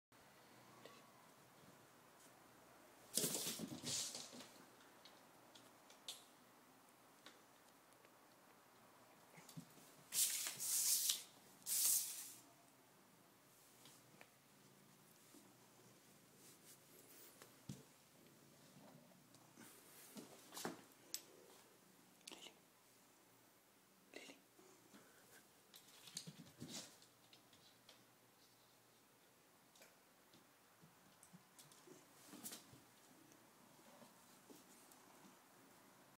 Cat playing catch with a stick
My cat brought me this stick and I threw it down the hall, she brought it back right away. So then I threw it more and she always brought it back... smart kitty!